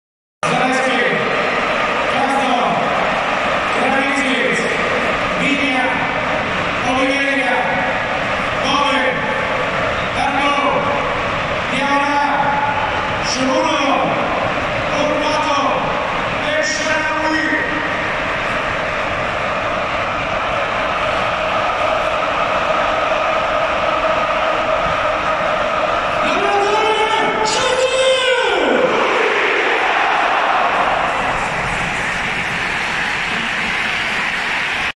14/5/2022 Roma Venezia: 70mila voci scandiscono il nome di José Mourinho
14 maggio 2022 Roma Venezia, il boato al nome di Mourinho